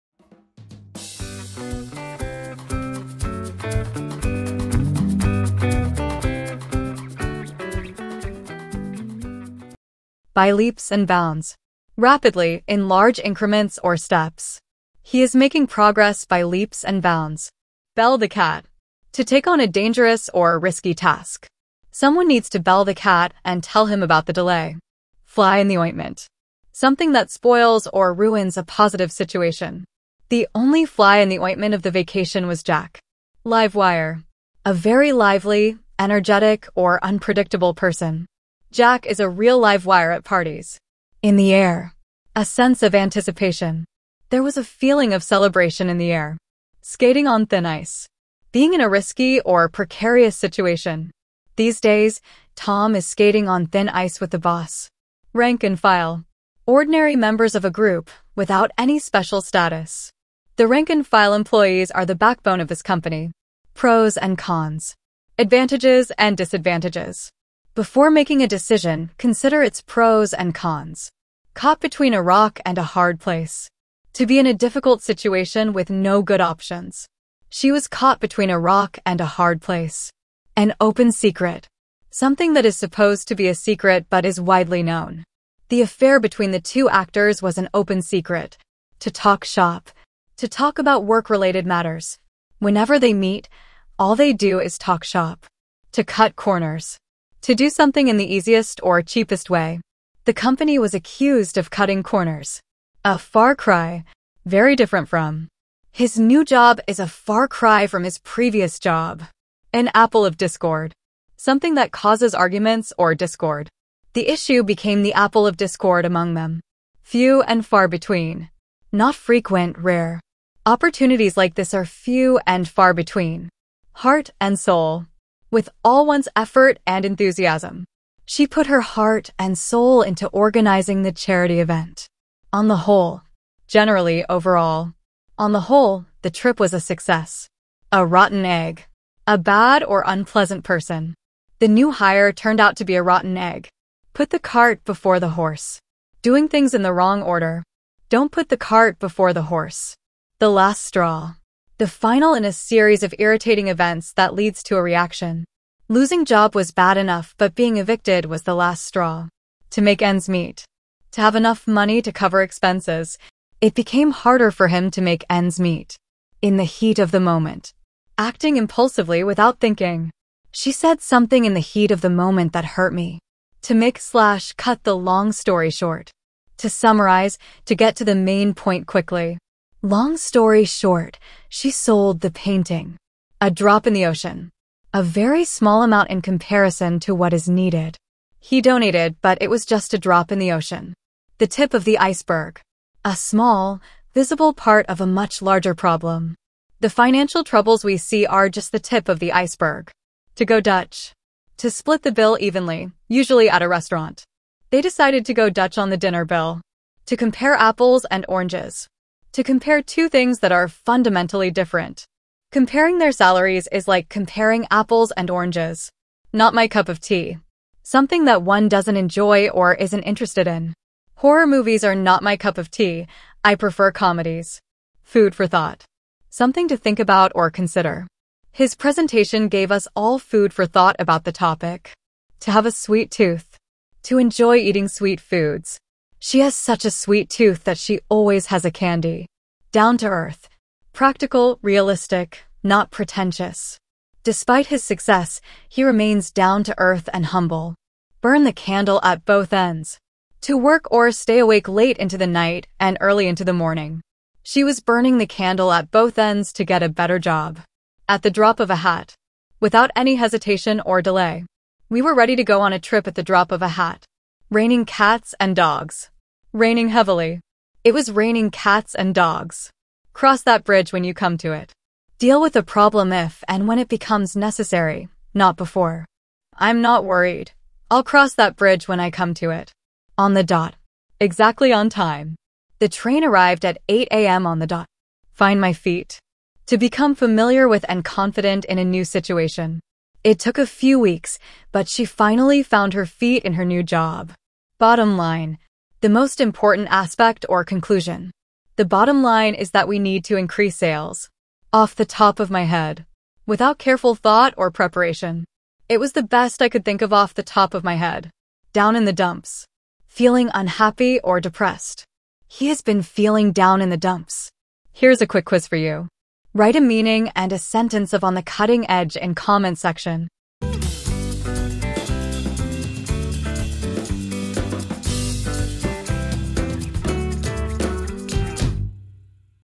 Episode 5 of Idiomatic phrases| Ielts preparation
Understanding and using idiomatic phrases holds significant importance both in the IELTS exam and daily life. In the IELTS exam, idioms showcase a higher level of language proficiency, demonstrating a nuanced grasp of English. They elevate writing and speaking responses, making them more engaging and sophisticated. Moreover, examiners look for candidates who can employ idiomatic expressions naturally and appropriately, enhancing the overall impression of fluency. Beyond exams, idioms enrich daily communication by adding color and depth to conversations. They facilitate clearer expression of thoughts and ideas, making interactions more lively and memorable. Mastering idioms helps individuals connect with native speakers on a deeper level, fostering better relationships and cultural understanding. In essence, idiomatic phrases are essential tools for effective communication in both academic and everyday settings.